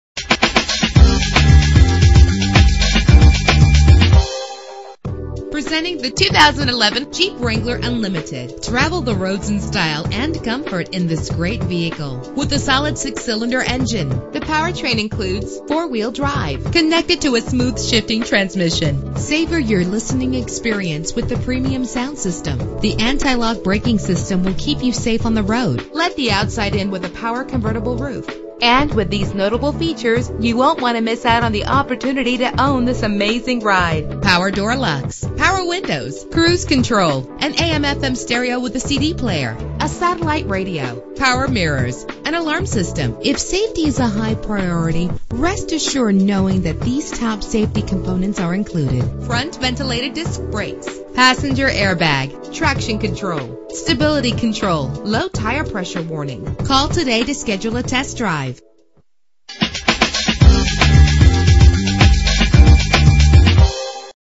2011 Jeep Wrangler Unlimited - Lafayette LA
Year: 2011
Make: Jeep
Model: Wrangler Unlimited
Trim: Sahara
Engine: 3.8 liter 6 cylinder 12 valve
Transmission:
Color: Silver
Mileage: 0
Address:
1700 S.E. Evangeline Thruway